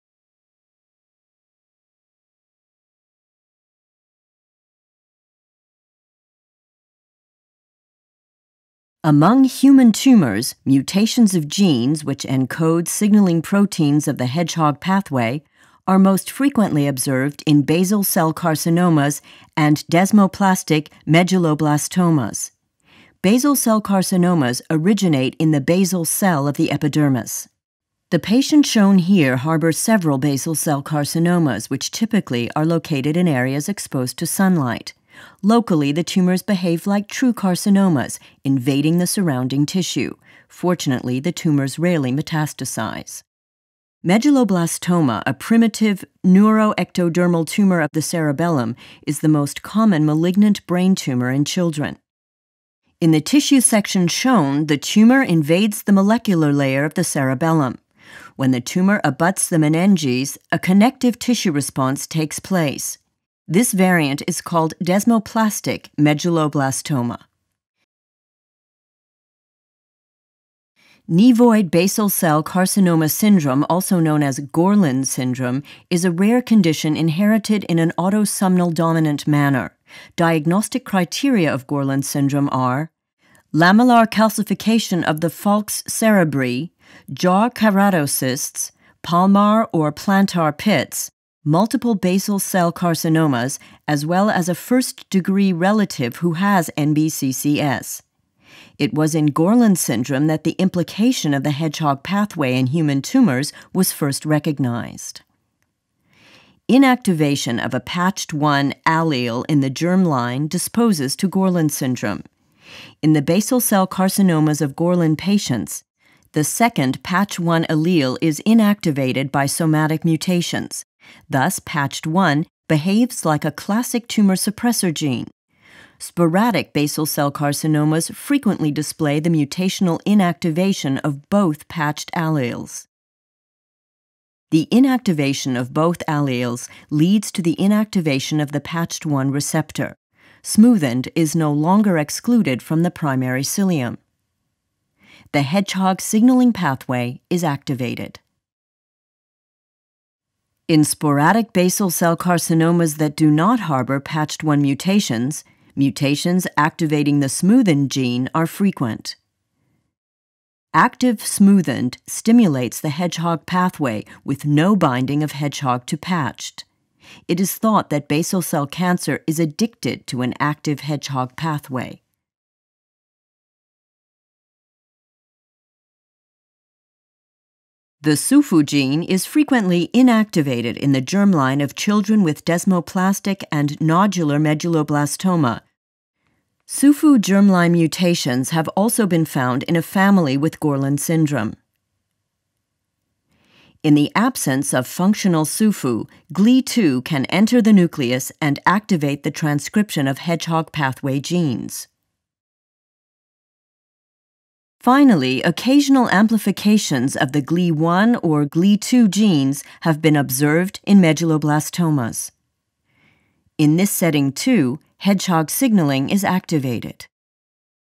The hedgehog signaling pathway – Part 2
The hedgehog signaling pathway – Part 2: Activation of the Hedgehog pathway in basal cell carcinoma and medulloblastoma

The hedgehog signaling pathway is constitutively active in cells of basal cell carcinoma and medulloblastoma. This activation is caused by the mutational inactivation or the deletion of the Patched-coding gene PTCH or by activating mutations in the gene SMO, which codes for Smoothened. Other mutations include inactivating mutations in the gene that codes for Sufu, which prevents Gli from entering the nucleus, as well as activating mutations in the genes coding for Gli1 and Gli2.